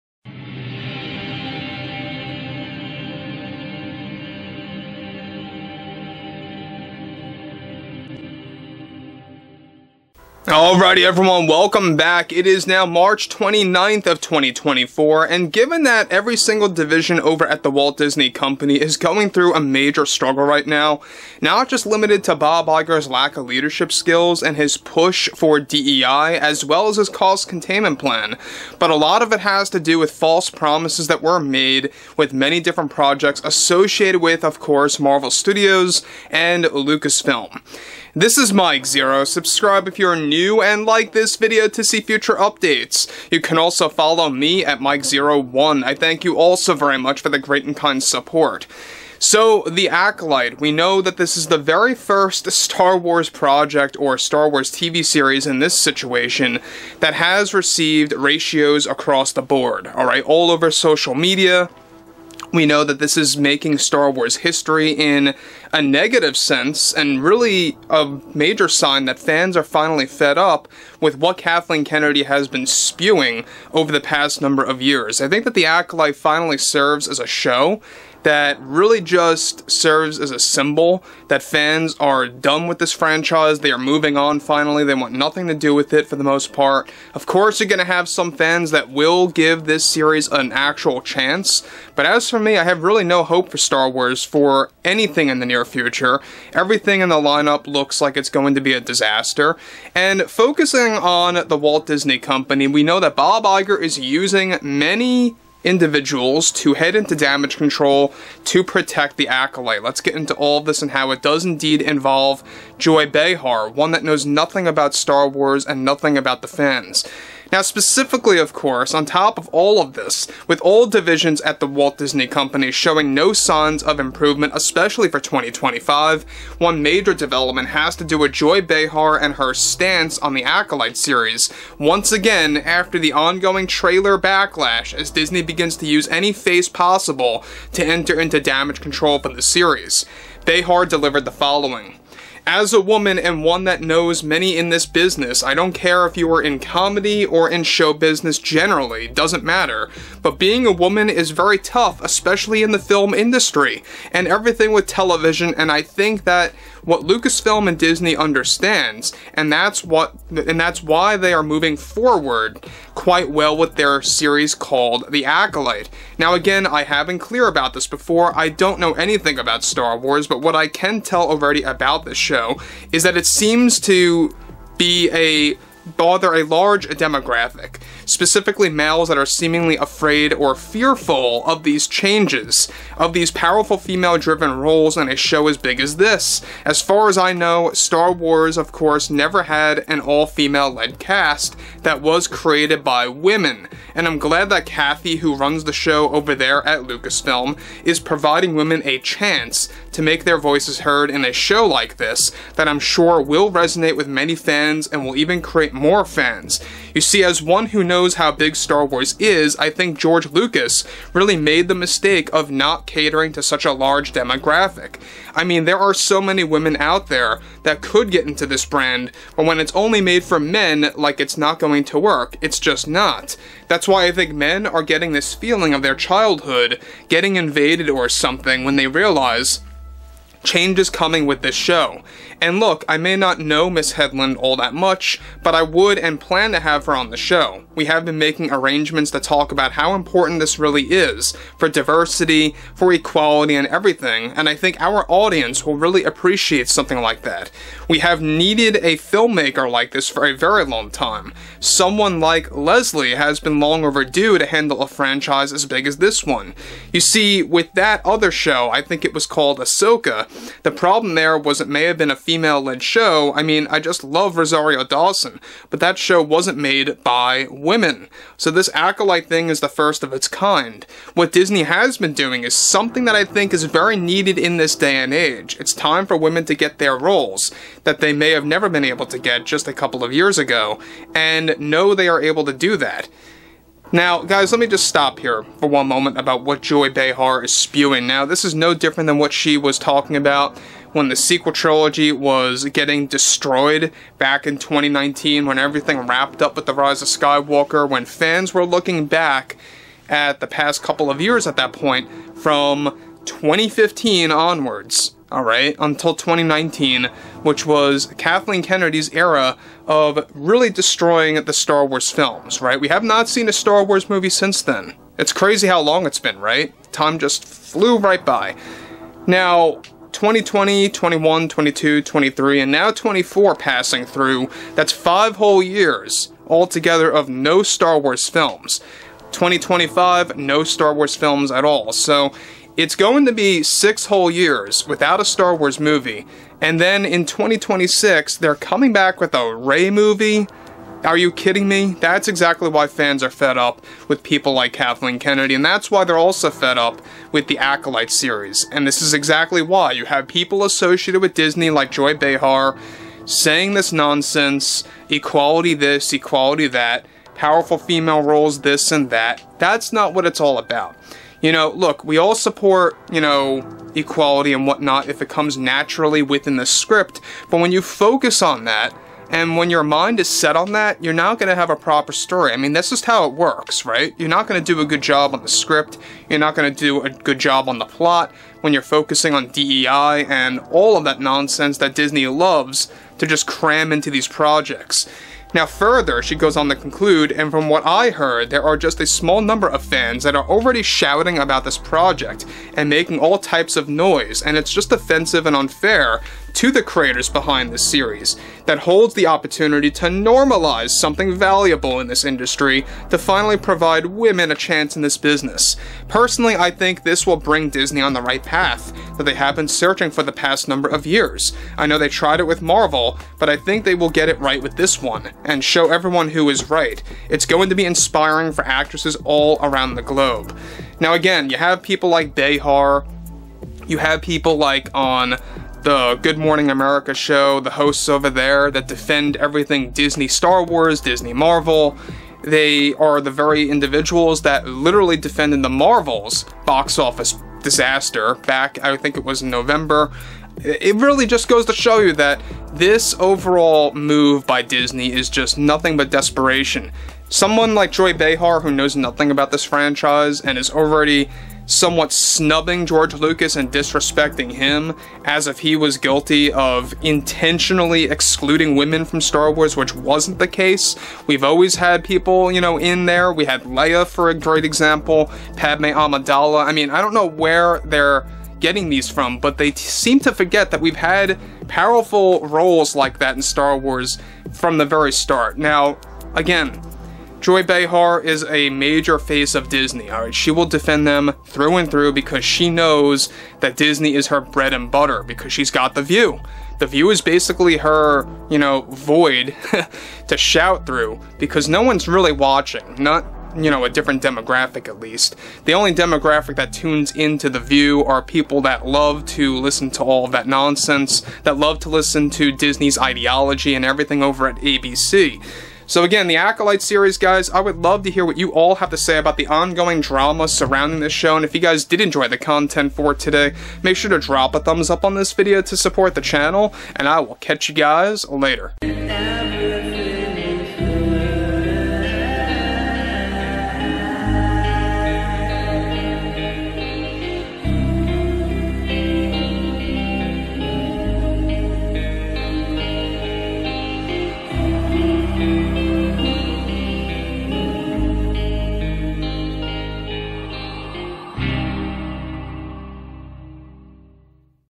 JOY BEHAR ATTACKS AFTER THE ACOLYTE TRAILER BACKLASH! DISNEY STAR WARS IS AN ABOMINATION
With the Acolyte trailer backlash only getting worse for those like Bob Iger, Kathleen Lennedy and especially Leslye Headland. all of lucasfilm is beginning to head into damage control after the acolyte trailer disaster hits an all time low for disney star wars. one major development has to do with Joy Behar of the view and her stand against the fans over the acolyte backlash. This all comes in the wake after how the Media attacks Star Wars Fans After The Acolyte Gets DESTROYED and is an All Time WORST For Disney Star Wars for sure! Already kathleen kennedy is also heading into damage control over the acolyte series for disney plus that is set to ruin the star wars prequels by george lucas.